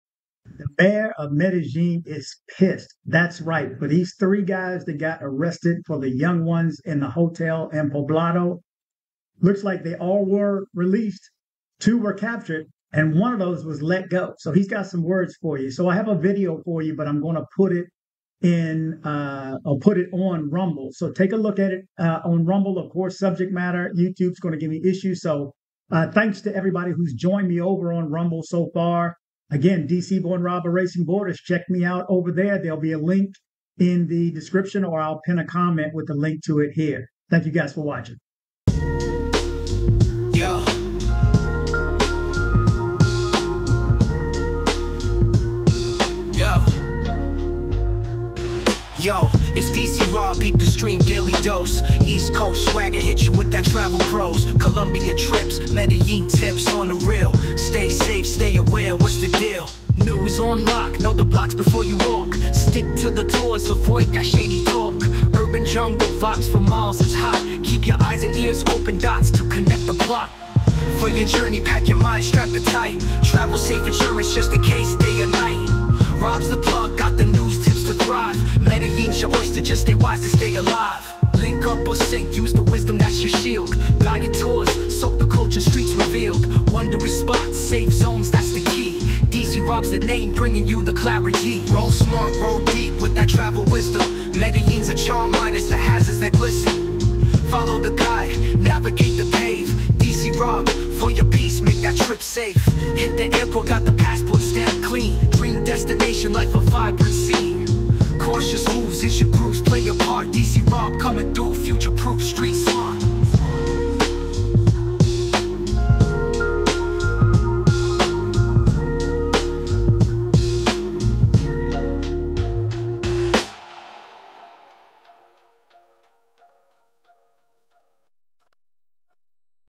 Mayor Outraged as Foreigners Accused of Exploiting Minors in Medellin Are Freed
Mayor Outraged as Foreigners Accused of Exploiting Minors in Medellin Are Freed. In a shocking update from Medellin, Mayor Federico Gutiérrez has expressed deep frustration over the release of three foreign citizens accused of sexually exploiting minors in Parque Lleras. Despite the initial arrests, the justice system set them free, and only one has been recaptured. The mayor has been vocal about his disappointment, particularly after learning that the Venezuelan suspect was released due to a supposed lack of full identification. Gutiérrez is calling for real justice, demanding that the rights of rapists should not come before the safety of children. The American suspects are still being pursued, with one captured at Bogotá's El Dorado airport while attempting to flee to the U.S. Watch the full video to learn more about this controversial case and the mayor's fight for justice.

If you'd like to donate to either my channel or my charity, please use my PayPal Business link. It's on my channel page